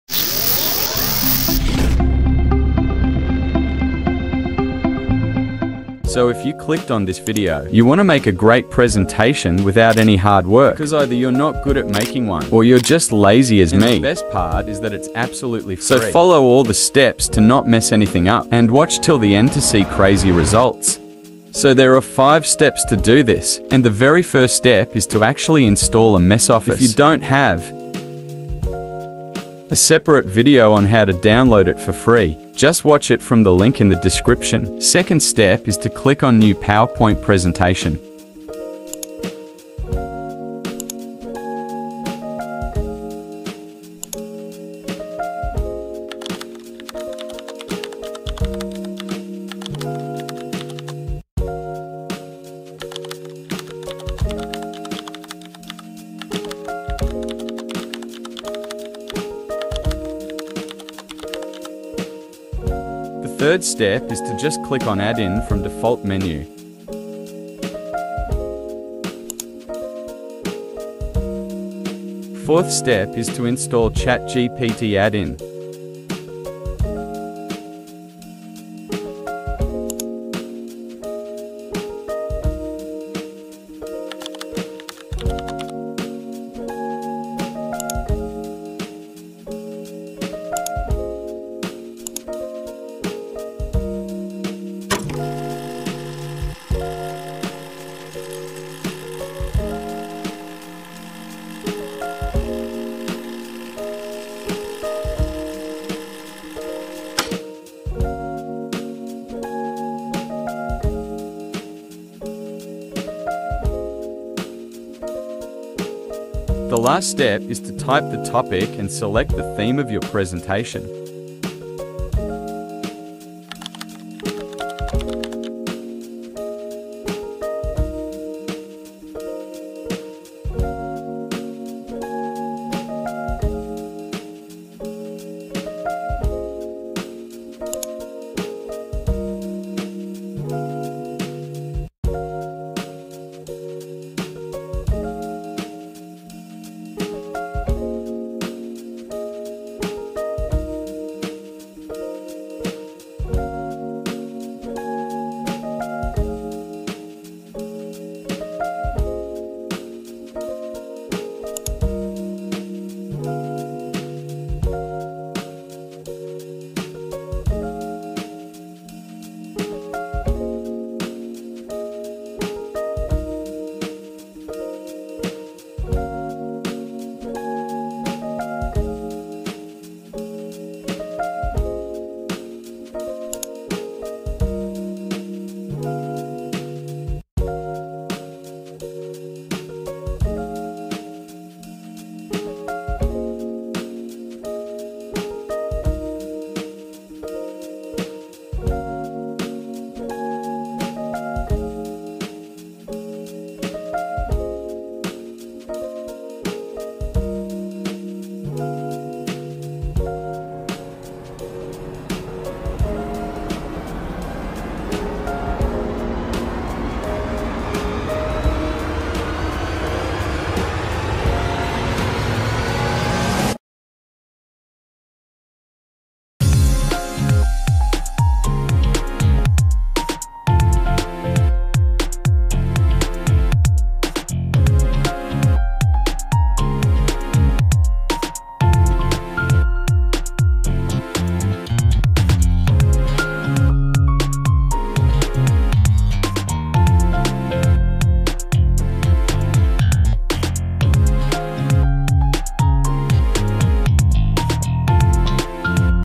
How to Create PowerPoint Presentations for Free with ChatGPT
Link to download office for free

Learn how to create stunning PowerPoint presentations for free using ChatGPT! This step-by-step tutorial shows you how to generate professional slides effortlessly, saving time and effort. Perfect for students, professionals, and anyone looking to simplify their presentation process.